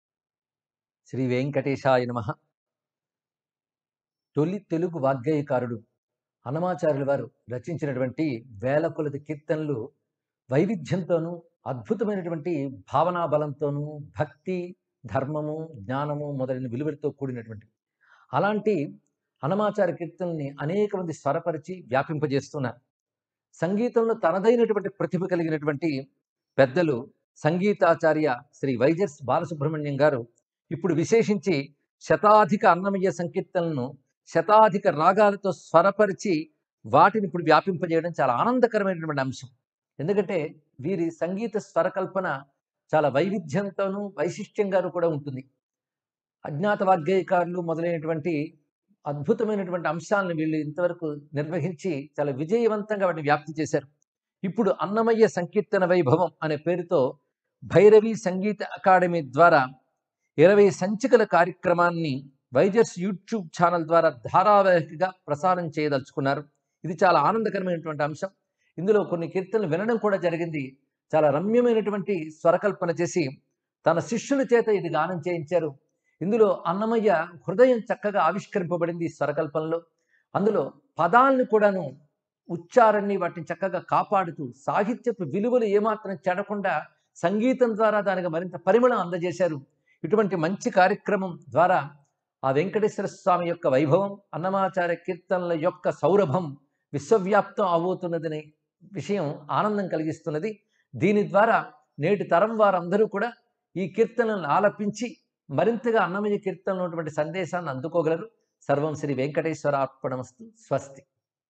Annamayya Sankeertana Vaibhavam - Blessings from Brahmasri Samavedam Shanmukha Sarma garu
Blessings and Good wishes from Brahmasri Samavedam Shanmukha Sarma garu for our upcoming series - Annamayya Sankeertana Vaibhavam